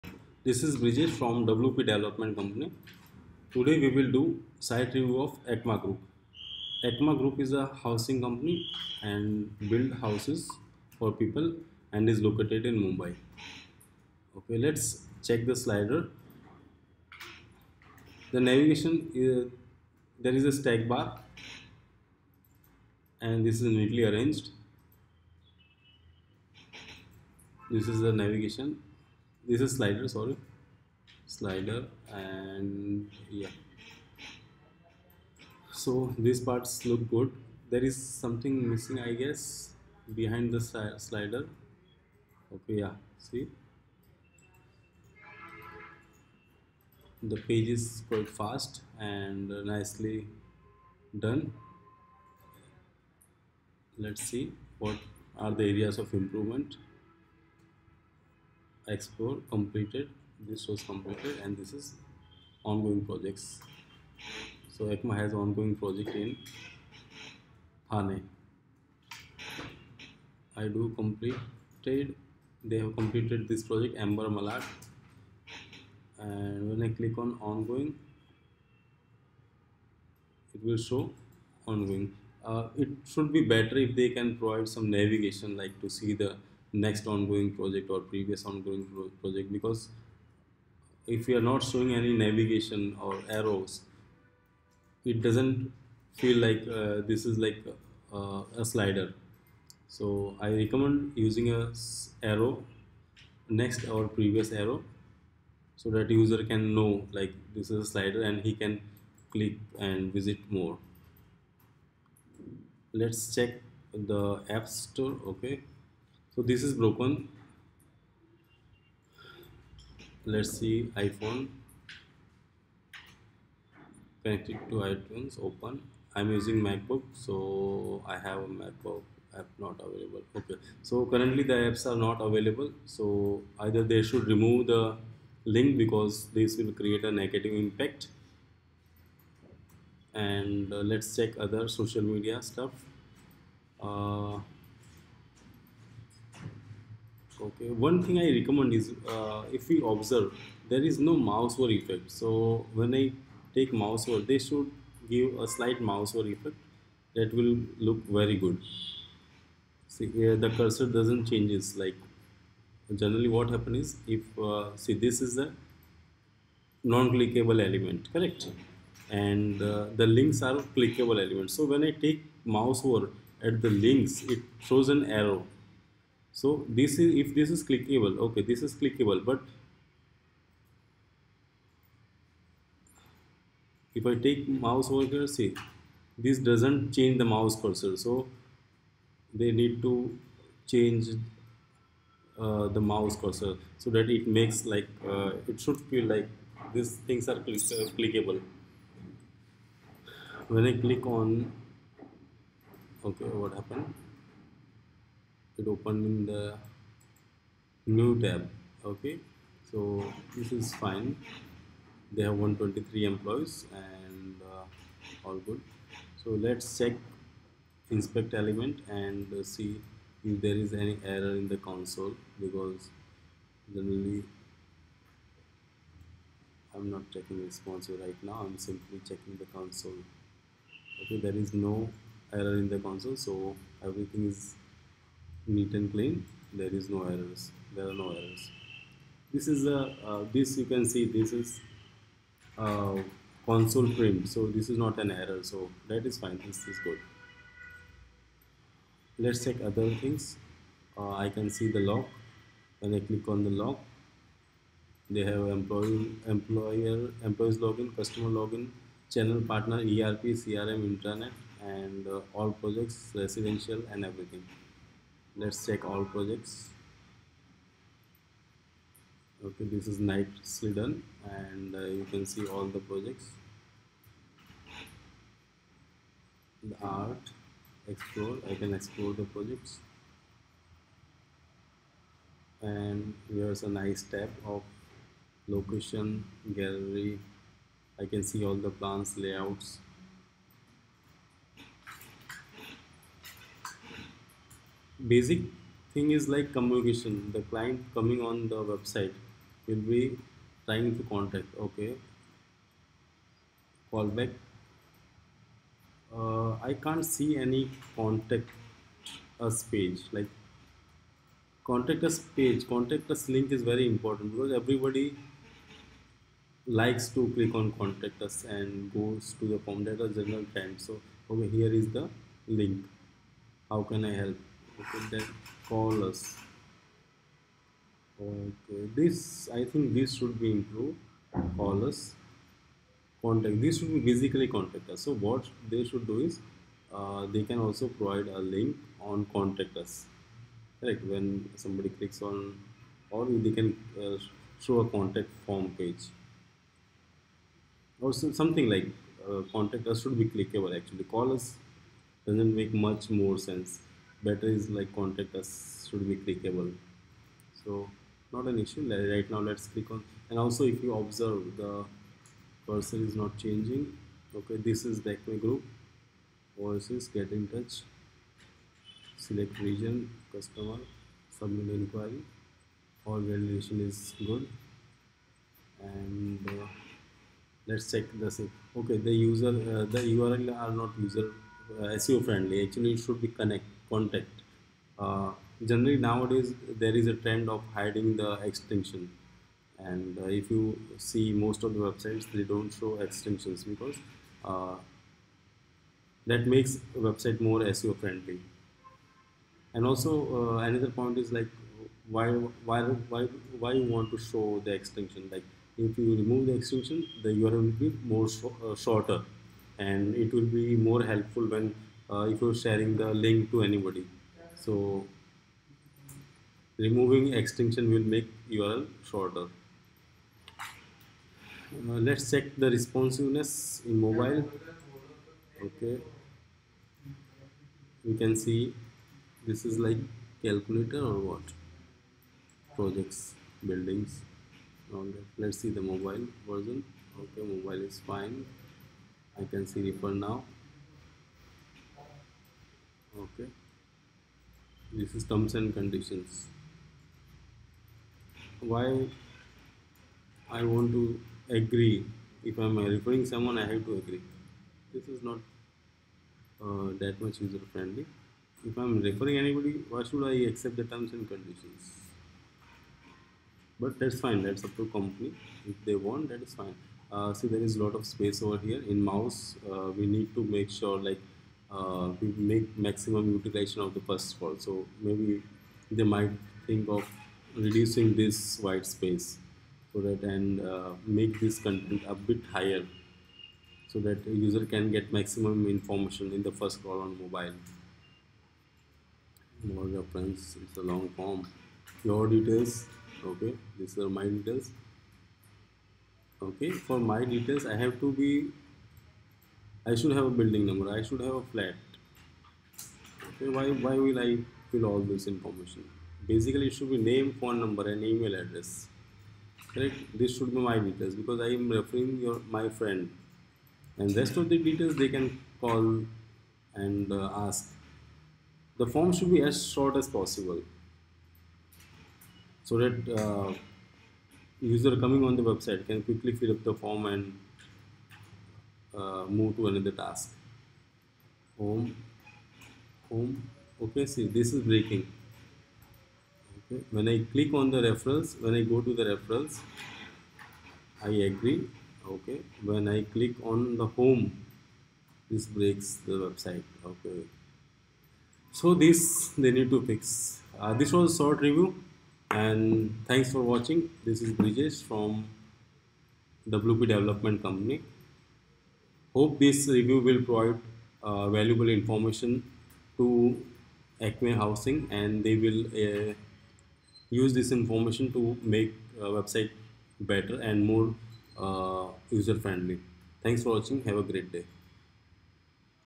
Review of acme group website by WP Development Company
Acme group is a housing company, located in Mumbai and has won many rewards.

Need technical help and support? Visit

We will be happy to help and assist you.

Regards,
Brijesh